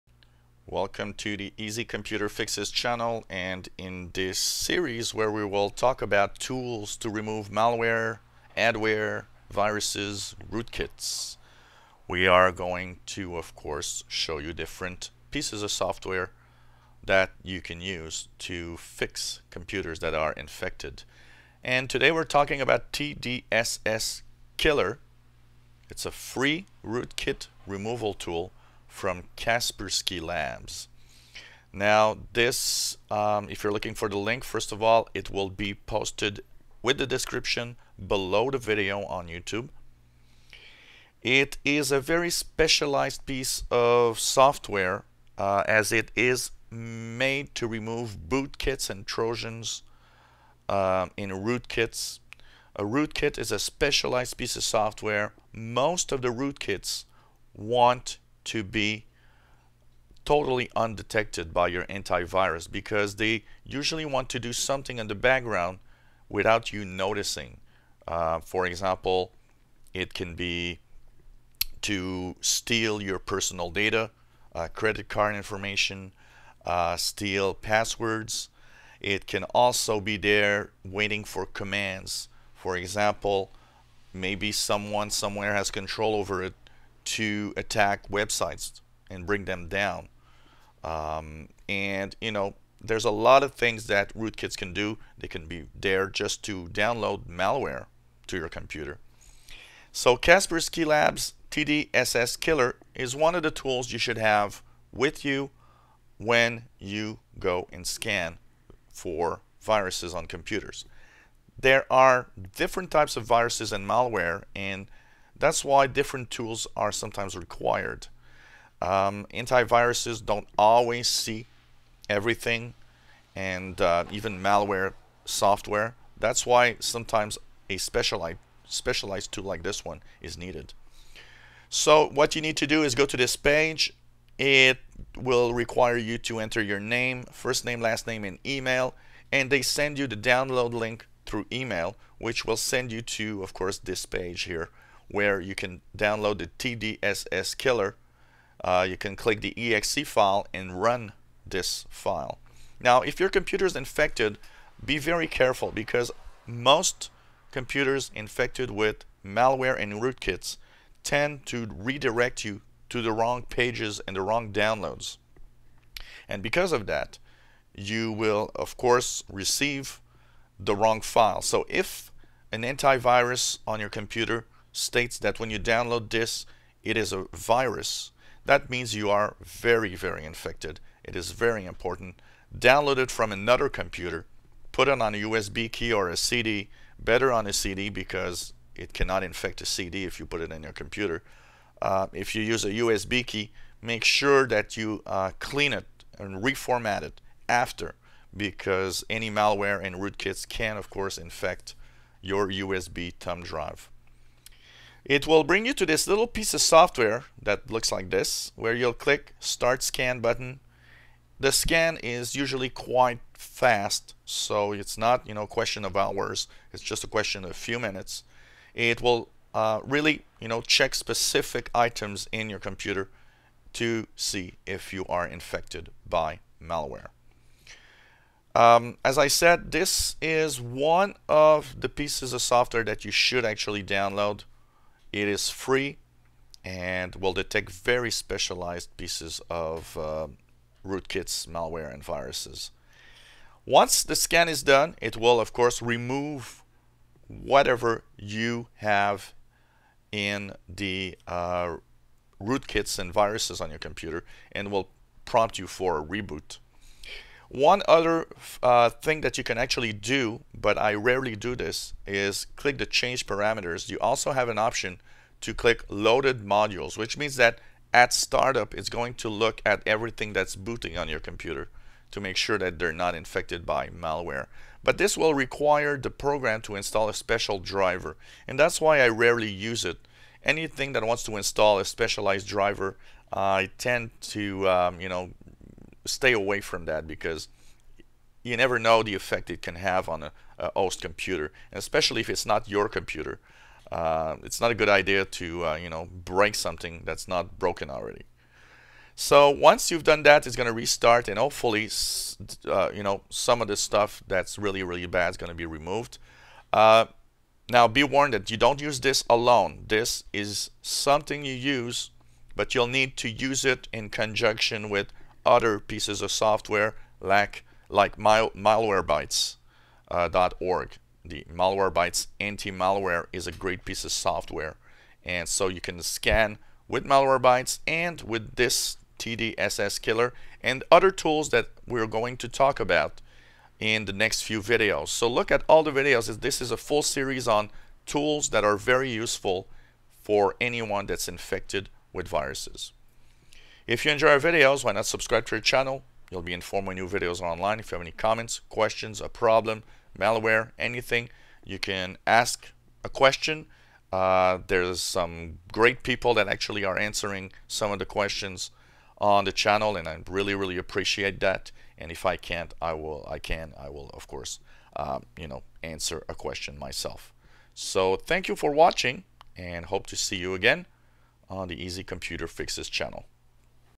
FIXIT Virus Malware and Rootkits removal 101 Kaspersky Lab TDSSKILLER utility
Using Kaspersky Lab TDSSKILLER to remove viruses malware and Rootkits in this series of videos on tools to remove malware on PC's